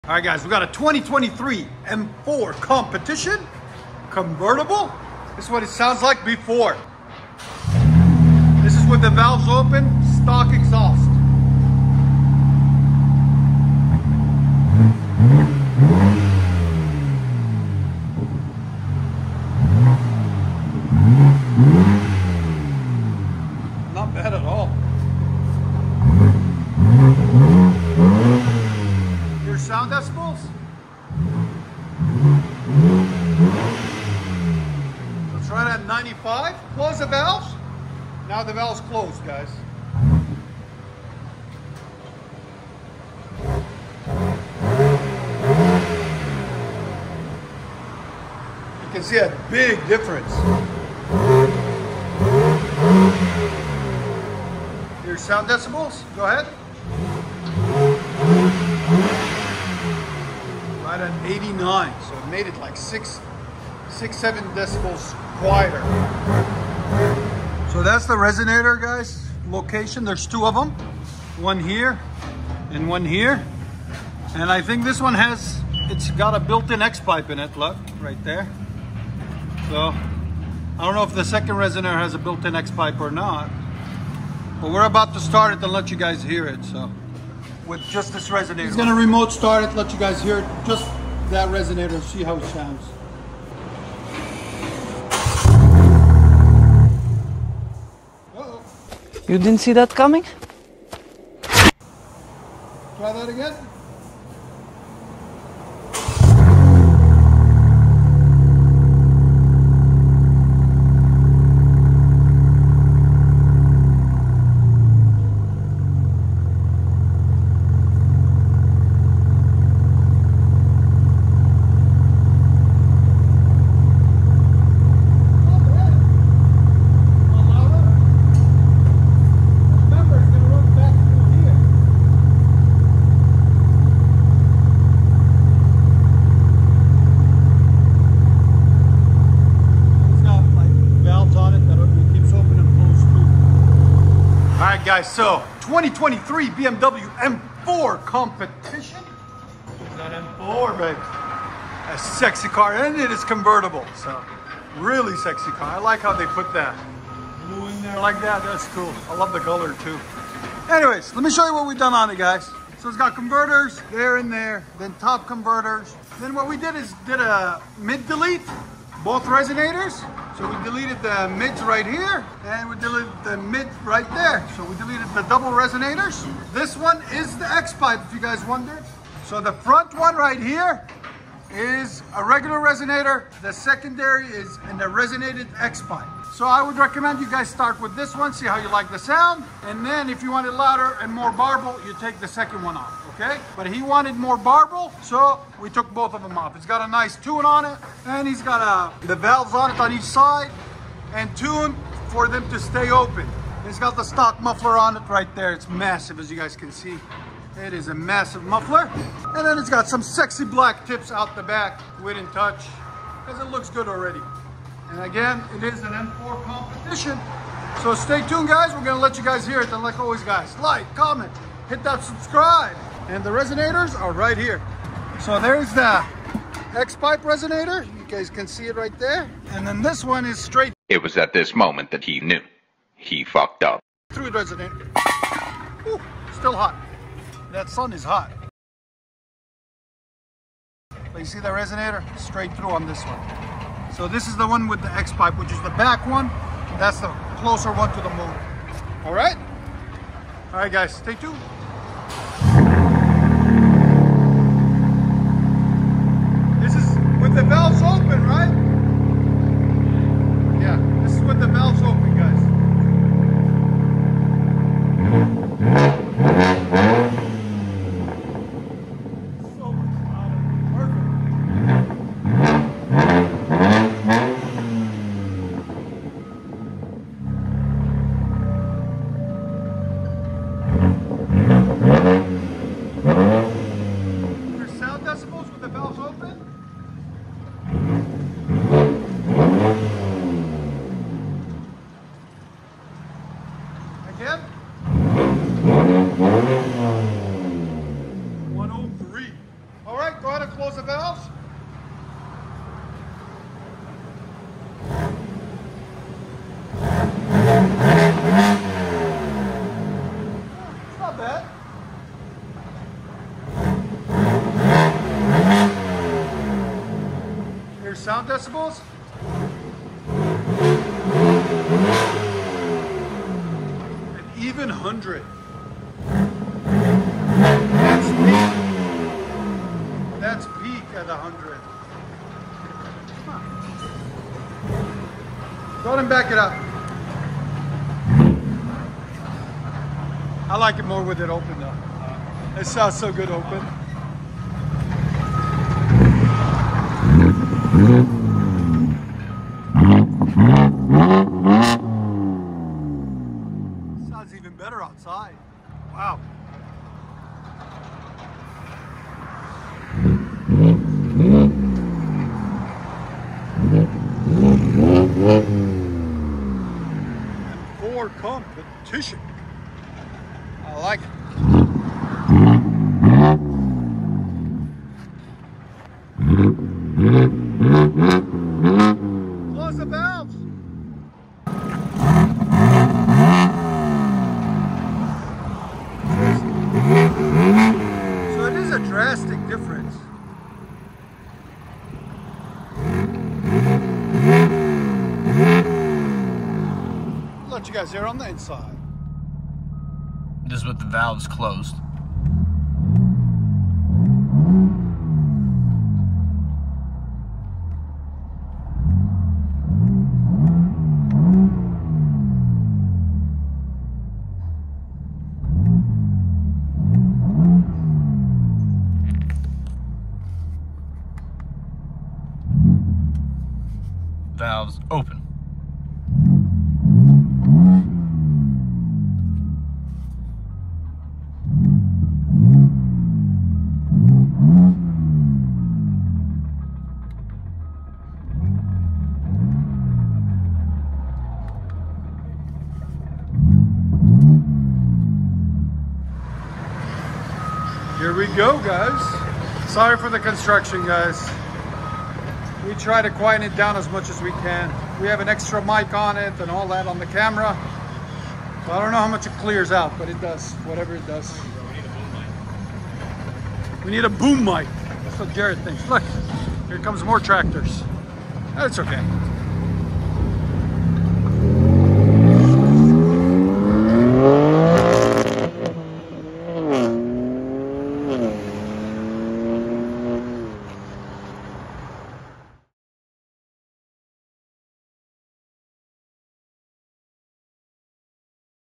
2023 BMW M4 Competition 3.0L TT I6 w/ X-PIPE DELETE!
2023 BMW M4 Competition 3.0L TT I6 w/ X-PIPE DELETE!

0:00 BEFORE: Stock Exhaust
1:07 X-Pipe Delete Test
2:24 Undercarriage
4:11 Decibel Reading & Revs
6:38 Interior Sound
7:26 Drive Off